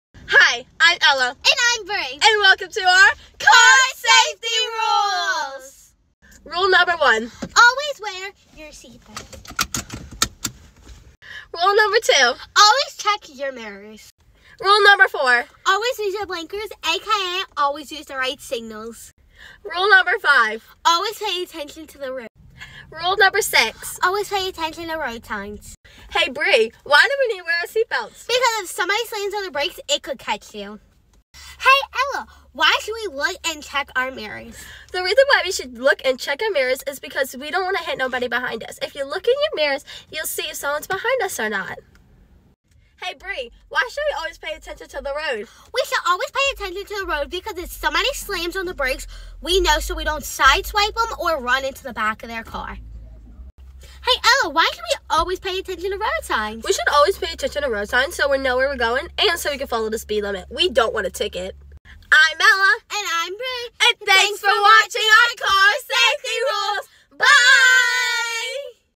3rd place 6-8th grade division: Ella and Bre Car Safety Rules - Breannah Long & Ella Loustalot
The 3rd place winner in the 6-8th grade division of Destination Zero Deaths' 2022 Thrive To Survive The Drive Distracted Driving Awareness Video Contest comes from Andrew Jackson Middle School in Chalmette, LA!

Directors: Breannah Long & Ella Loustalot
Script Writers: Breannah Long & Ella Loustalot
Cinematographers: Breannah Long & Ella Loustalot
Actors: Breannah Long & Ella Loustalot